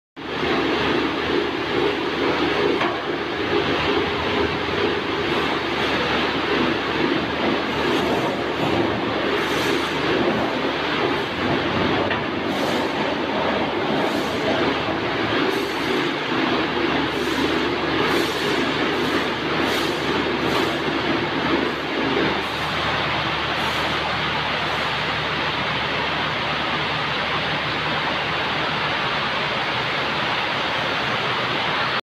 Manual H Beam Bending Machine I Beam Steel Profile Bending Machine
how to bend steel H beam